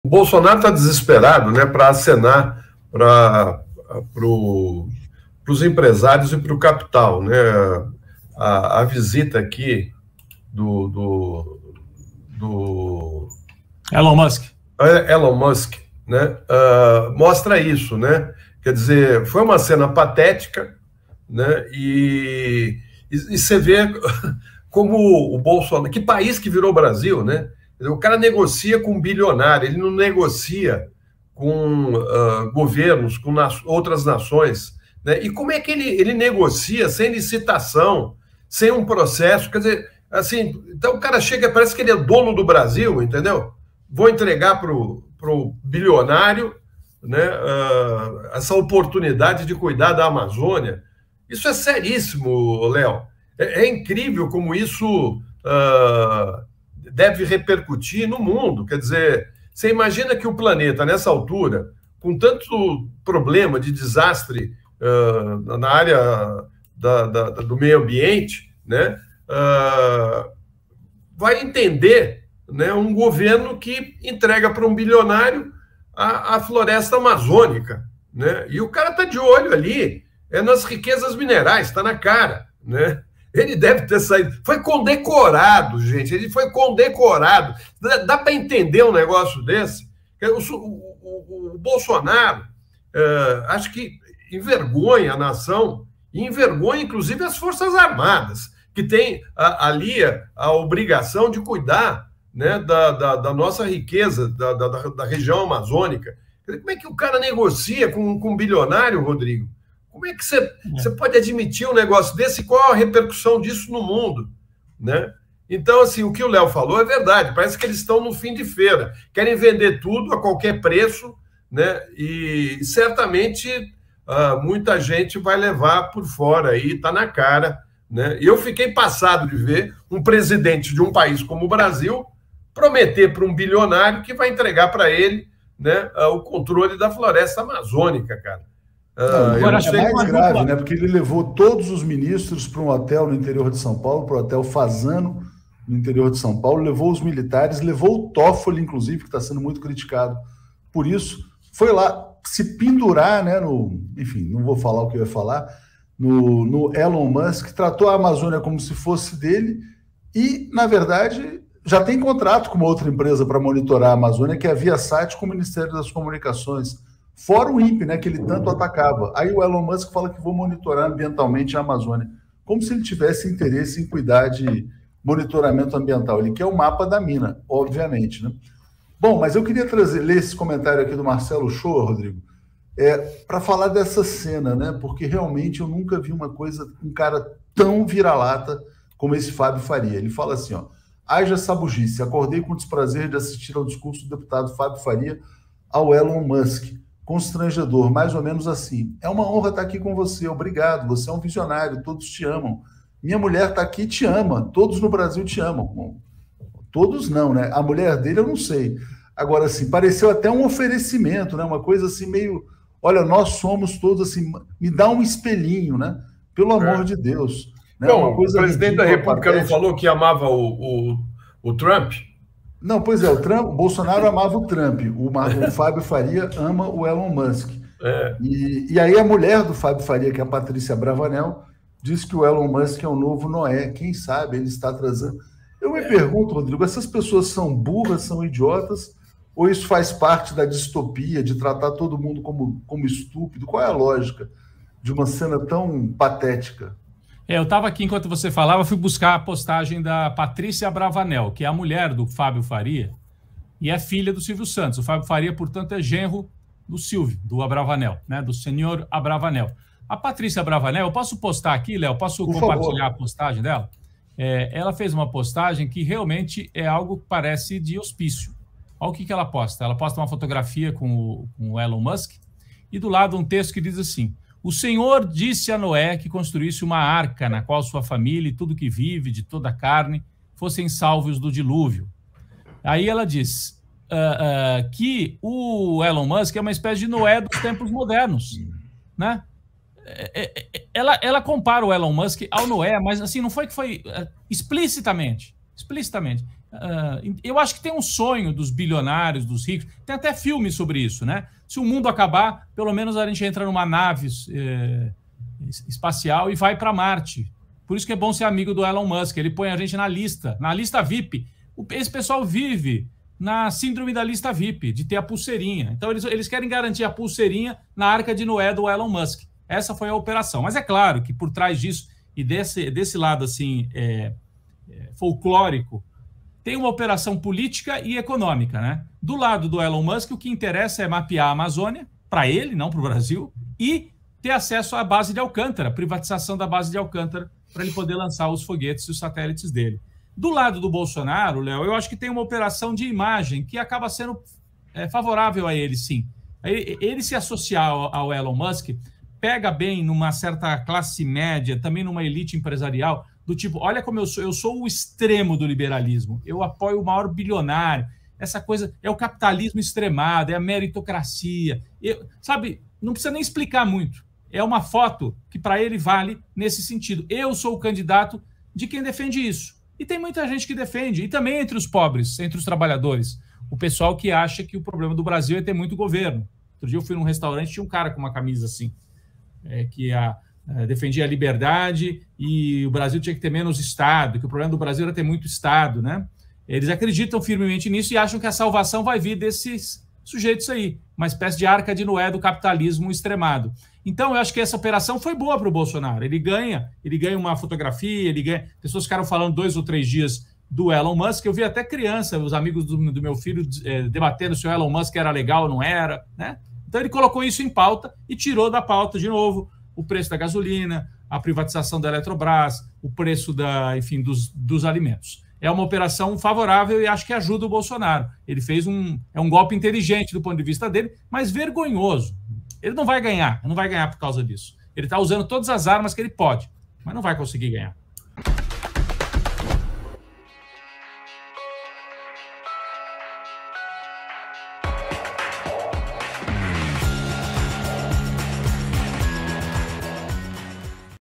"QUE PAÍS É ESTE QUE 'NEGOCIA' COM BILIONÁRIO E NÃO COM NAÇÕES?" | Cortes 247
Florestan Fernandes Júnior é jornalista, passou pelas principais redações e emissoras do Brasil, como Folha de S. Paulo, TV Globo, TV Cultura, TV Manchete.